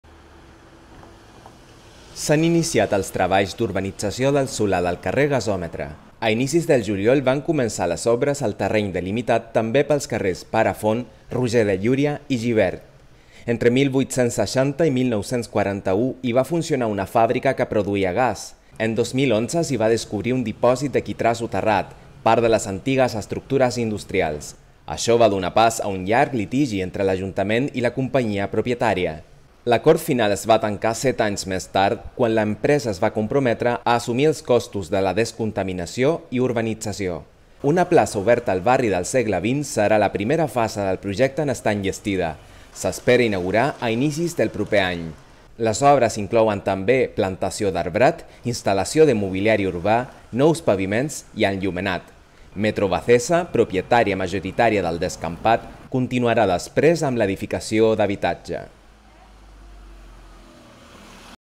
Comença la urbanització del solar del carrer Gasòmetre
S’han iniciat els treballs d’urbanització del solar del carrer Gasòmetre. A inicis del juliol van començar les obres al terreny delimitat també pels carrers Pare Font, Roger de Llúria i Gibert.